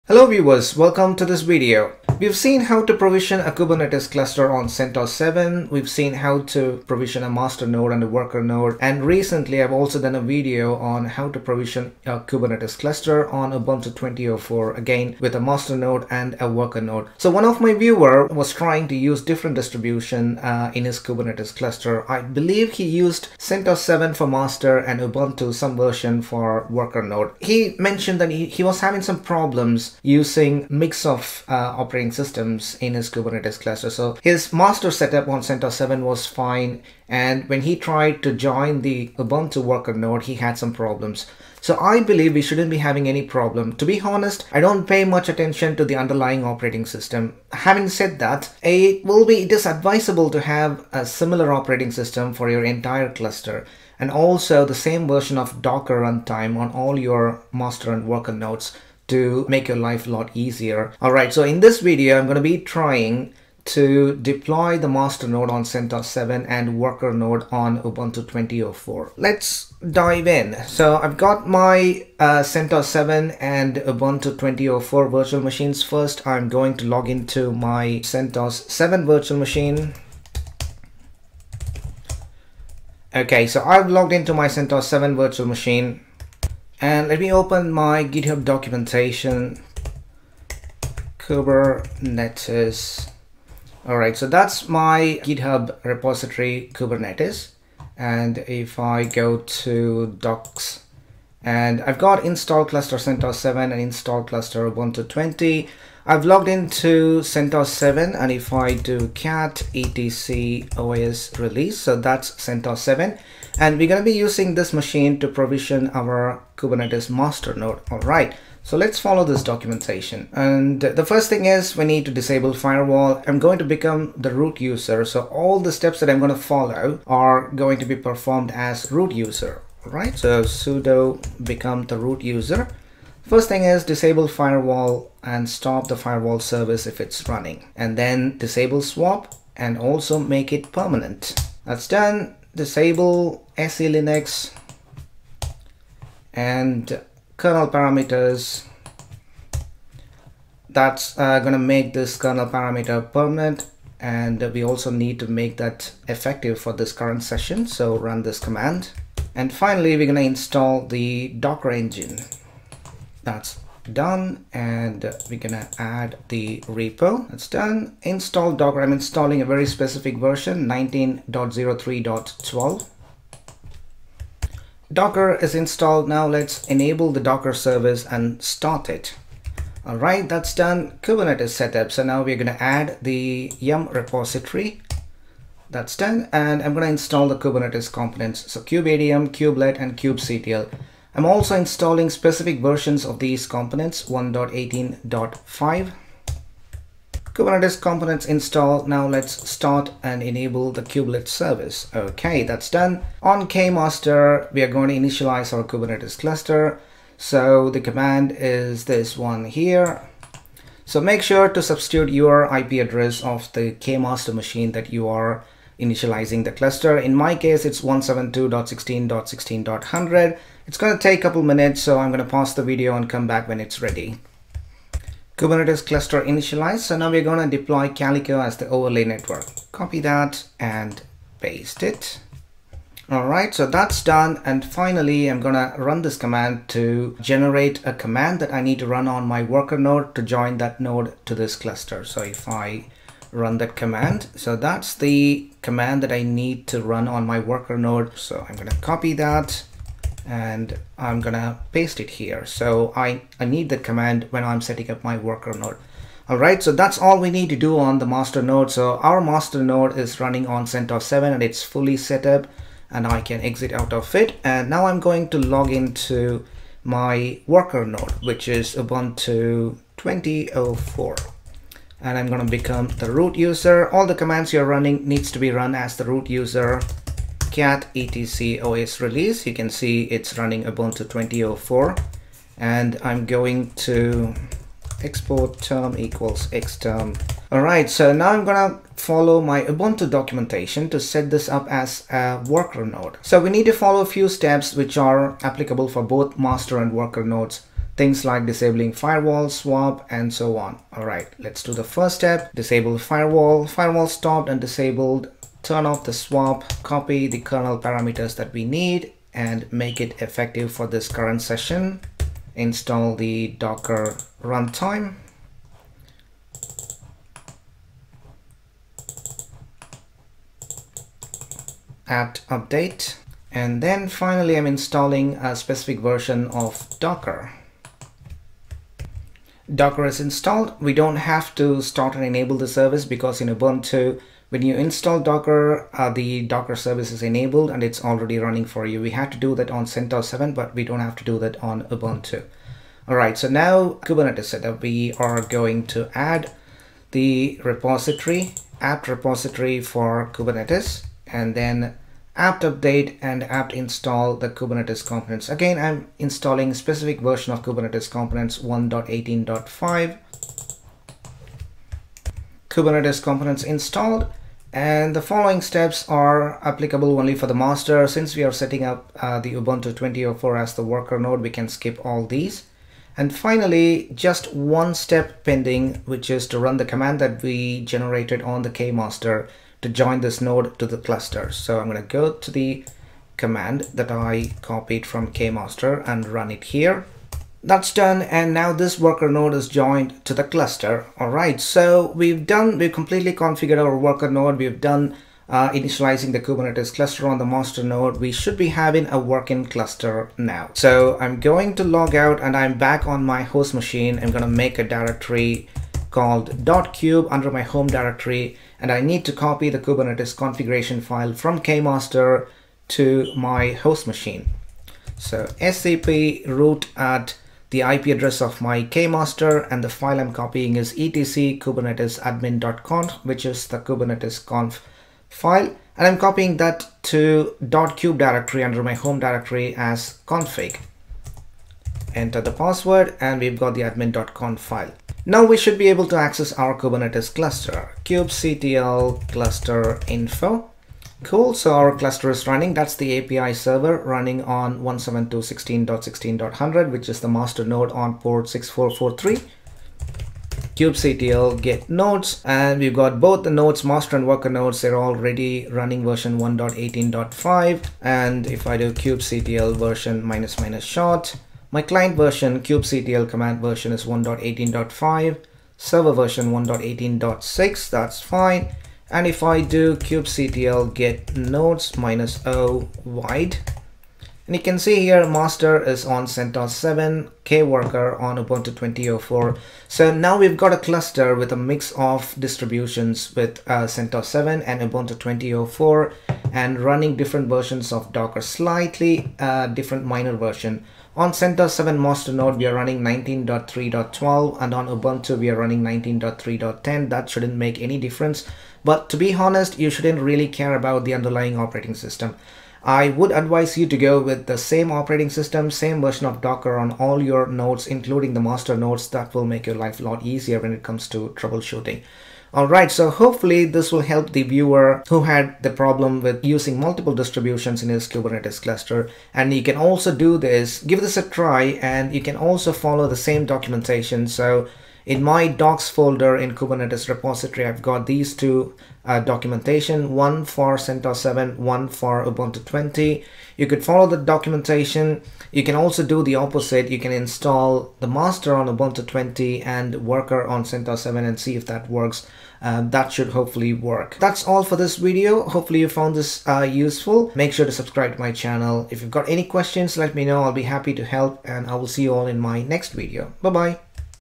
[ Kube 1.2 ] Setup Kubernetes Cluster on CentOS & Ubuntu
In this video I will show you how to provision a Kubernetes cluster using Kubeadm. I will be using CentOS 7 for master node and Ubuntu 20.04 for worker node.